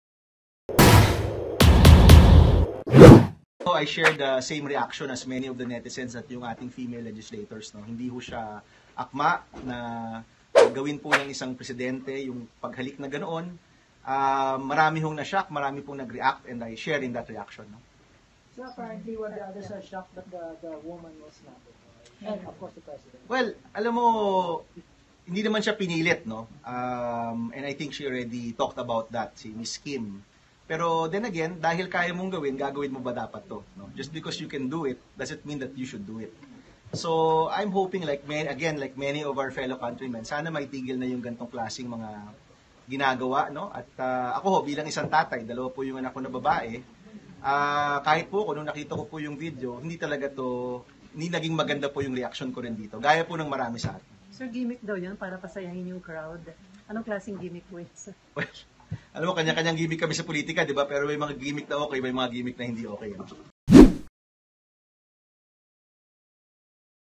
Hindi tamang manghalik si Duterte sa publiko - Bam
Hindi tamang manghalik si Pangulong Rodrigo Duterte sa publiko - Senator Bam Aquino